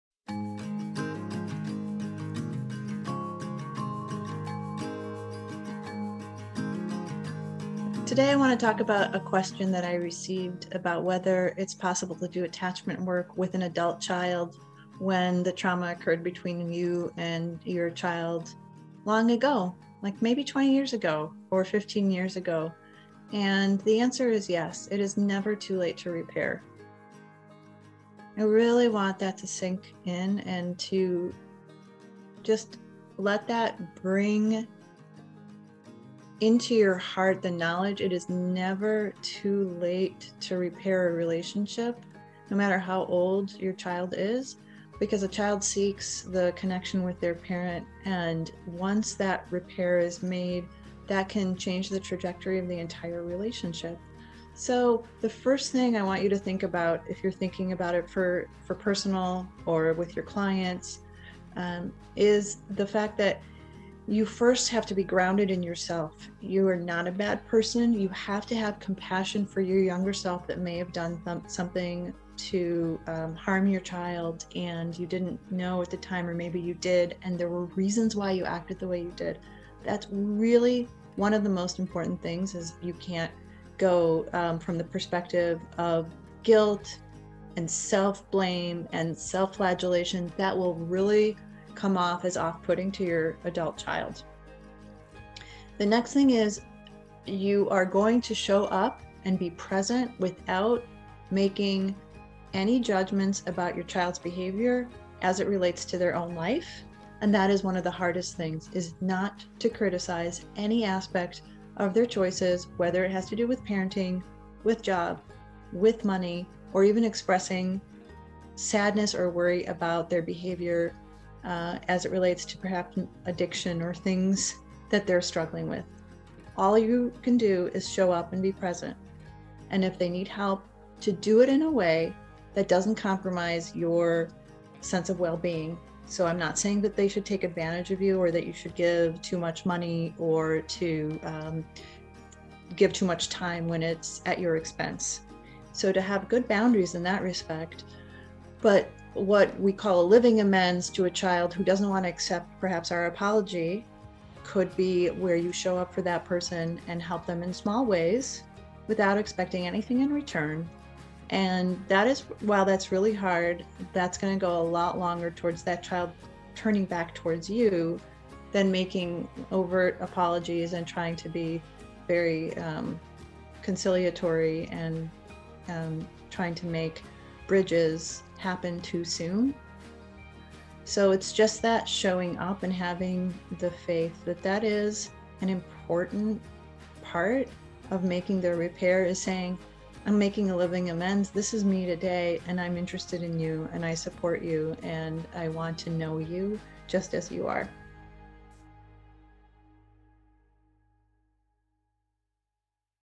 How to Make Amends with Adult Children
When we talk about relationships between parents and children, we often make the assumption that the children are still young and under their parents' care. For this week’s How To Tuesday, Dafna addresses how parents can repair relationships with adult children.